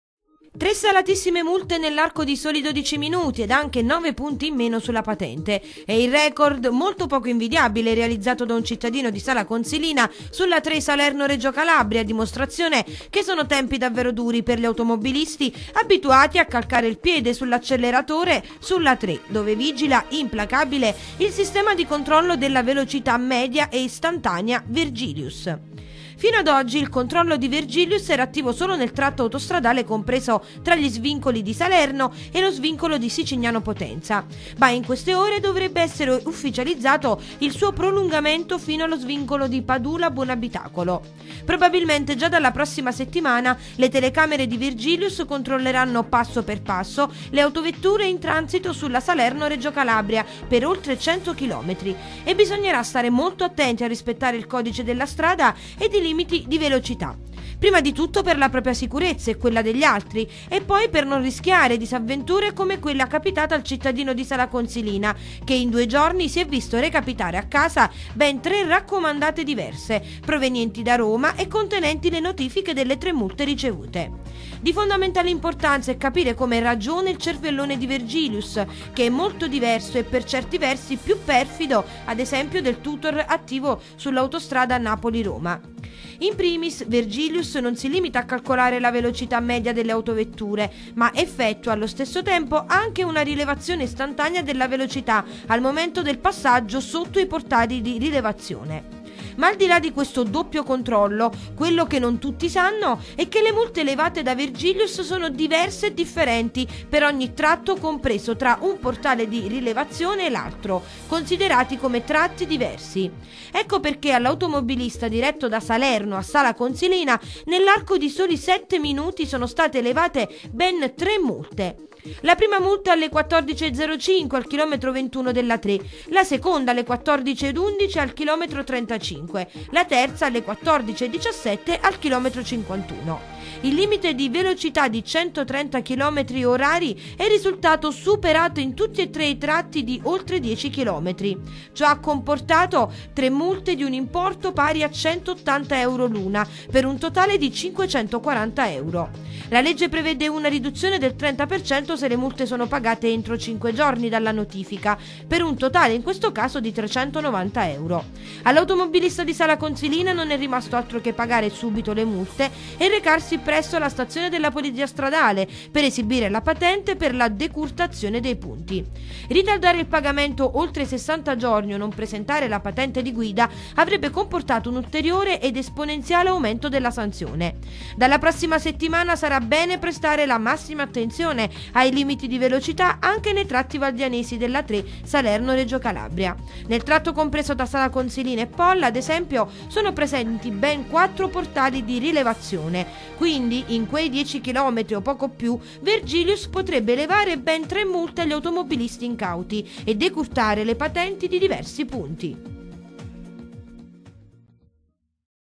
A3 SA/RC, record di multe da “Vergilius” ad un automobilista di Sala Consilina: 3 in 12 minuti!
08/11/2014